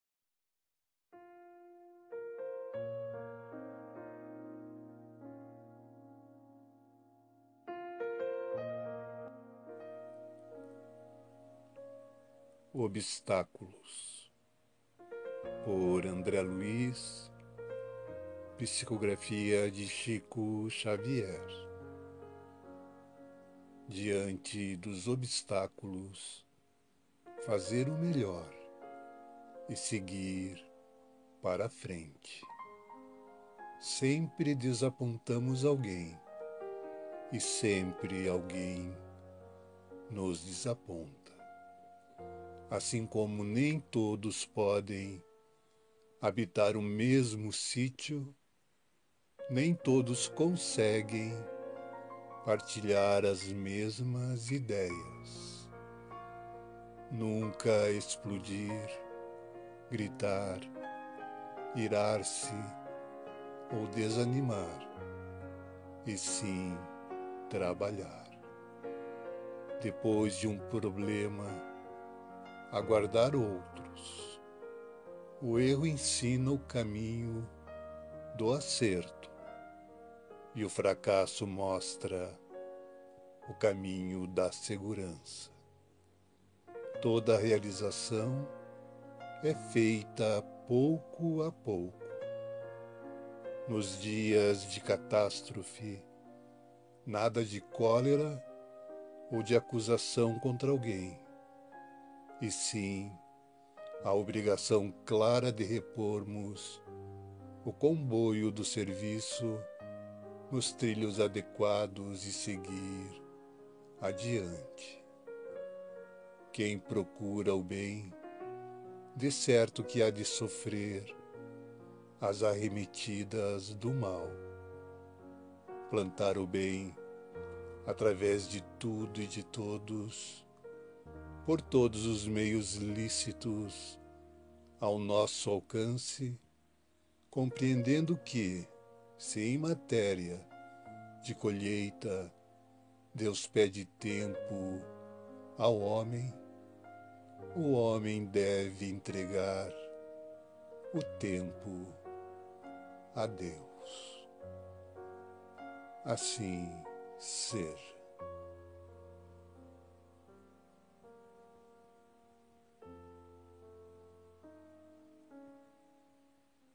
Obstáculos! André Luiz/Chico Xavier! ORAÇÕES E MENSAGENS PARA TODOS OS MOMENTOS!
Por: André Luiz, Do livro: Sinal Verde, Médium: Francisco Cândido Xavier.
Obstáculos
(André Luiz/Chico Xavier)
"Diante dos obstáculos, fazer o melhor e seguir para a frente.
Sempre desapontamos alguém e sempre alguém nos desaponta.
Assim como nem todos podem habitar o mesmo sítio, nem todos conseguem partilhar as mesmas ideias.
Nunca explodir, gritar, irar-se ou desanimar e sim trabalhar.
Depois de um problema, aguardar outros.
O erro ensina o caminho do acerto e o fracasso mostra o caminho da segurança.
Toda realização é feita pouco a pouco.
Nos dias de catástrofe, nada de cólera ou de acusação contra alguém, e sim a obrigação clara de repormos o comboio do serviço nos trilhos adequados e seguir adiante.
Quem procura o bem, decerto que há de sofrer as arremetidas do mal.
Plantar o bem, através de tudo e de todos, por todos os meios lícitos ao nosso alcance, compreendendo que, se em matéria de colheita Deus pede tempo ao homem, o homem deve entregar o tempo a Deus."